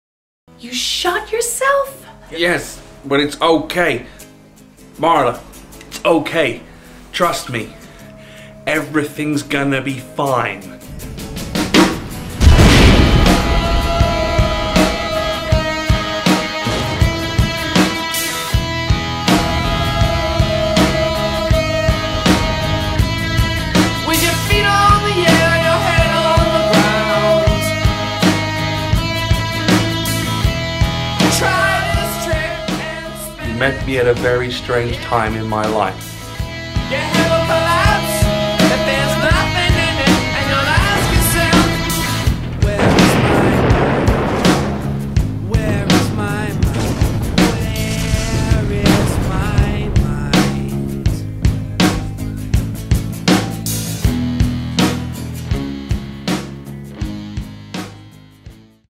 PERFECT MOVIE RECREATIONS: FIGHT CLUB
For Sarah Bennetto's Perfect Movie we recreated the final scene from Fight Club, which is her favourite ending to any film.